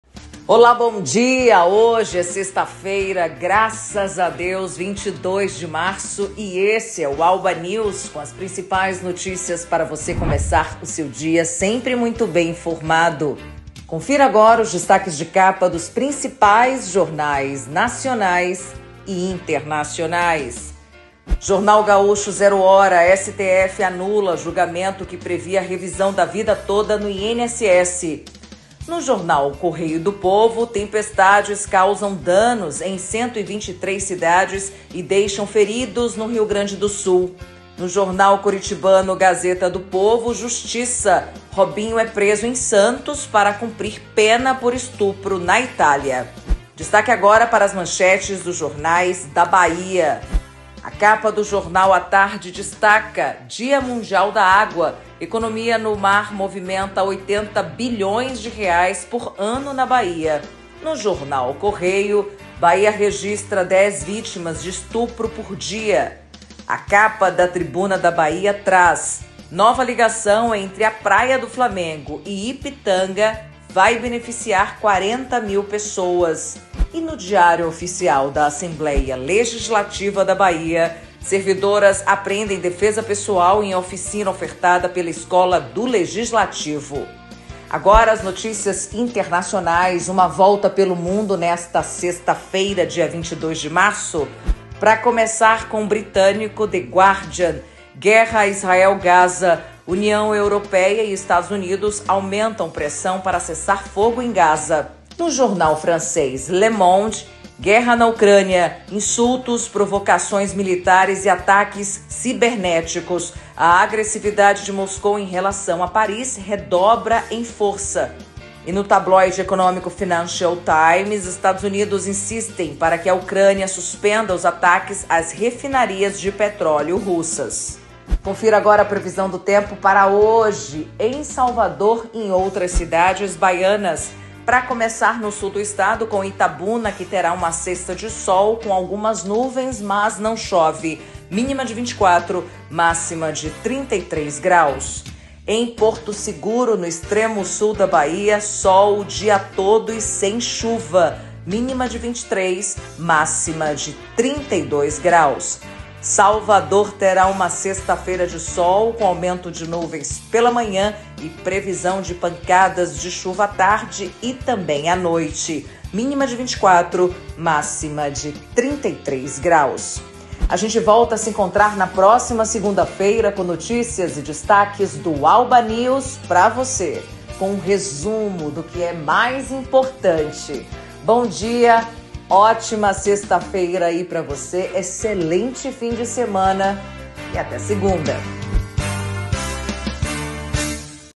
ALBA News 22/03/2024 - Principais manchetes dos jornais do dia
Robinho é preso em Santos para cumprir pena por estupro na Itália;
Economia do mar movimenta R$ 80 bi por
ano na Bahia;
Servidoras aprendem defesa pessoal em oficina ofertada pela Escola do Legislativo;
agora no ALBA News